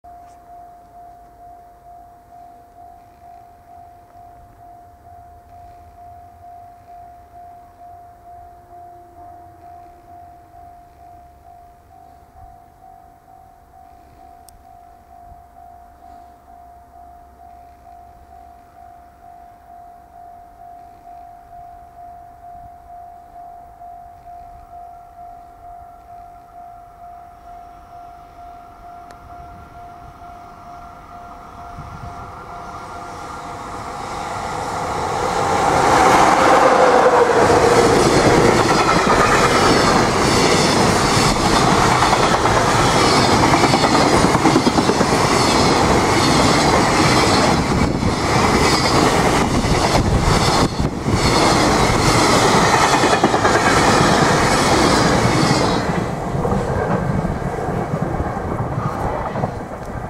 【復活!!】2014,4,13 貨物列車2070レ ゼロロクEF66-32号機牽引!!
岐阜～木曽川駅にて
この日は3年ぶりに復活を遂げたEF66-32号機が2070レ(岐阜タ～名古屋タ)を牽引しました!!
自分自身も32号機を撮影したのは3年ぶりでした!